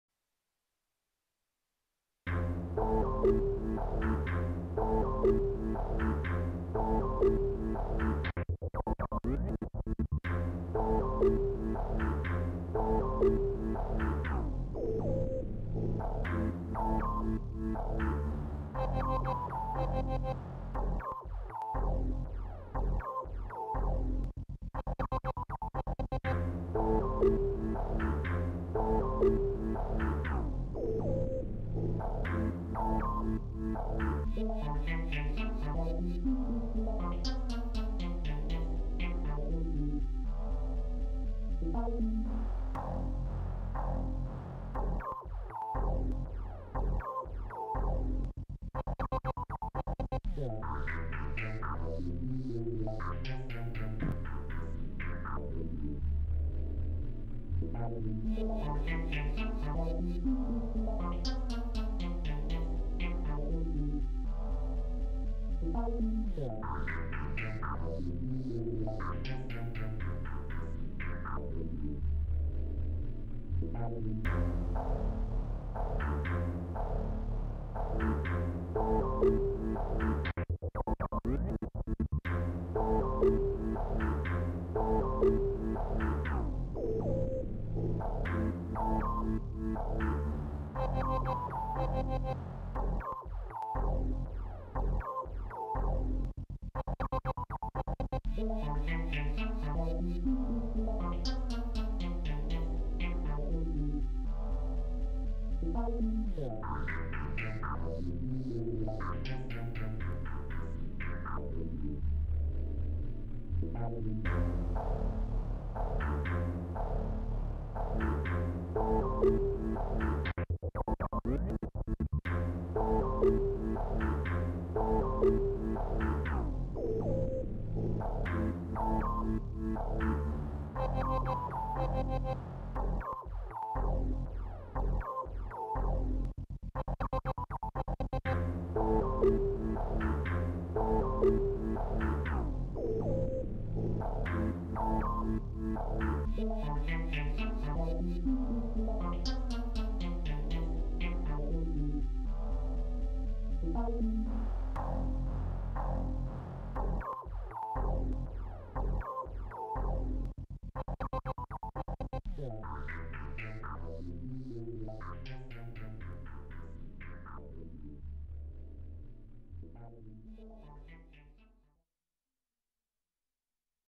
JAMuary2021 #07 (PO-33)
Yeah, uploaded just a few minutes late.  But I started recording before midnight, so I’m considering my streak continued!  😜

Same samples on the PO-33 from the last video.

I think this is C-minor.  120bpm